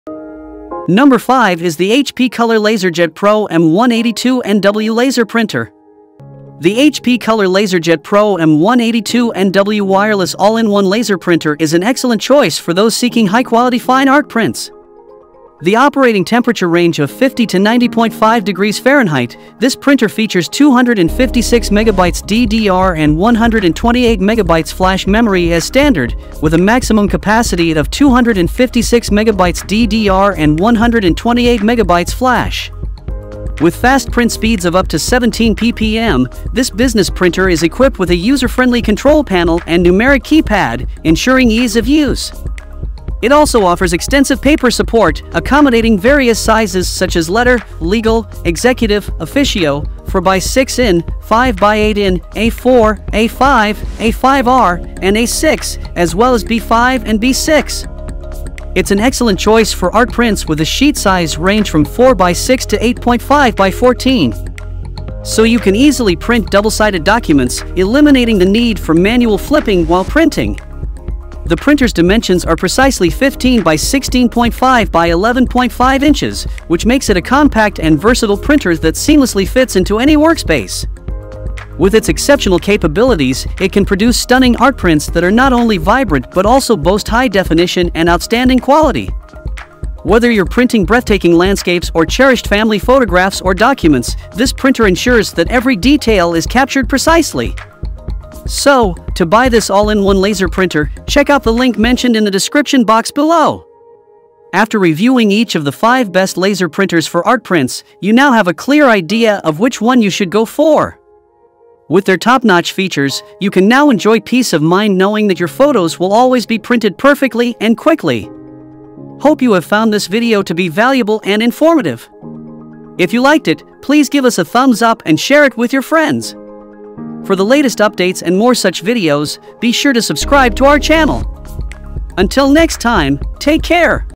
HP Color LaserJet Pro M182nw | All-in-One Laser Printer | 2024 Updates
If you need a laser printer that offers high-quality color and fast multifunction versatility while also supporting a plethora of paper types without compromising on security, the HP Color LaserJet Pro M182nw stands out artistically. It’s not just a printer; it’s a comprehensive creative hub for artists seeking precision and convenience. With print, copy, and scan capabilities at speeds of up to 17 ppm, this printer streamlines your workflow. The HP Smart app elevates the experience, allowing remote printer setup, job management, and on-the-go printing and scanning, providing ultimate flexibility for your artistic pursuits. Customizable shortcuts save valuable time by simplifying repetitive tasks, while the printer’s strong security measures safeguard both the device and your sensitive data, ensuring peace of mind while you create. The HP Color LaserJet Pro M182nw’s wireless connectivity offers reliable performance through dual-band Wi-Fi, ensuring a stable connection for uninterrupted printing sessions. Plus, with Original HP Toner cartridges featuring JetIntelligence, expect professional-quality prints every time, backed by innovative anti-fraud technology. This printer’s commitment to excellence extends to its wide range of supported paper sizes, accommodating various artistic formats. Backed by a one-year limited hardware warranty and 24/7 web support, it’s a reliable partner for your artistic endeavors. Elevate your artistry with the HP Color LaserJet Pro M182nw—a fusion of performance, security, and versatility.